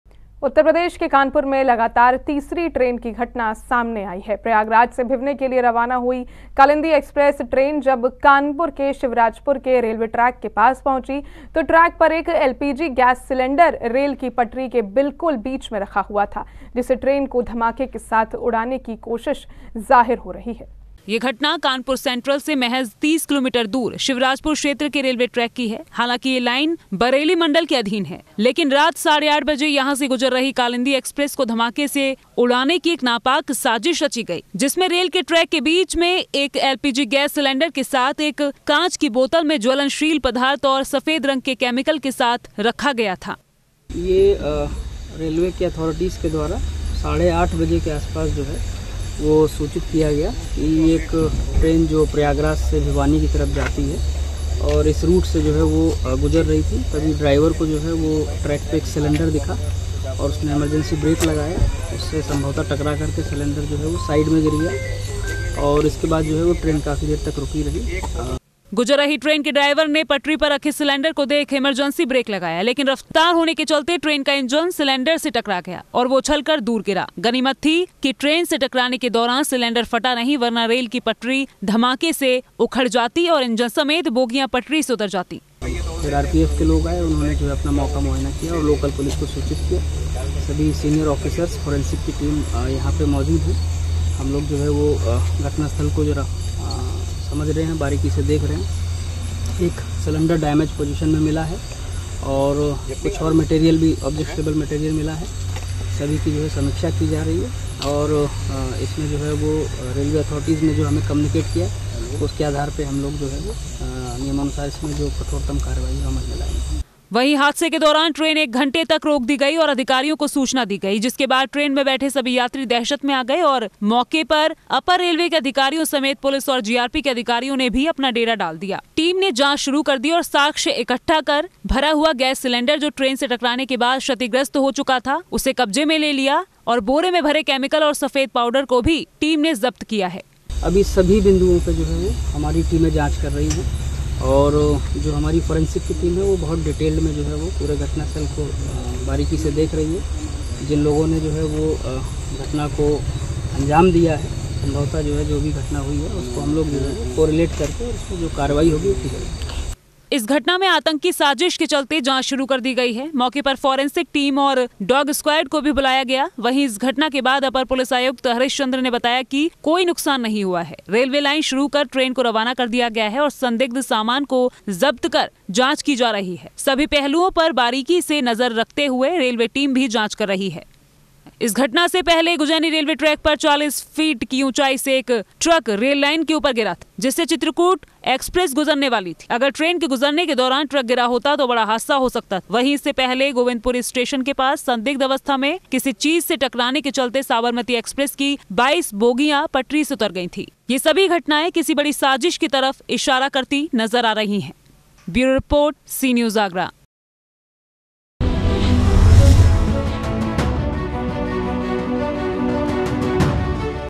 Kanpur: कालिंदी एक्सप्रेस को डिरेल करने की साजिश | Train Incident | Kalindi Express train | Sea News
उत्तर प्रदेश के कानपुर में लगातार तीसरी ट्रेन की घटना सामने आई है, इसमें हर बार इसे साजिश के तहते चलती ट्रेन को नुकसान पहुंचाने का प्रयास किया गया है. प्रयागराज से भिवनैं के लिए रवाना हुई कालिंदी एक्सप्रेस ट्रेन जब कानपुर के शिवराजपुर के रेलवे ट्रैक के पास पहुंची तो ट्रैक पर एक एलपीजी गैस सिलेंडर रेल की पटरी के बिलकुल बीच में रखा हुआ था. जिसे ट्रेन को धमाके के साथ उड़ाने की कोशिश जाहिर हो रही है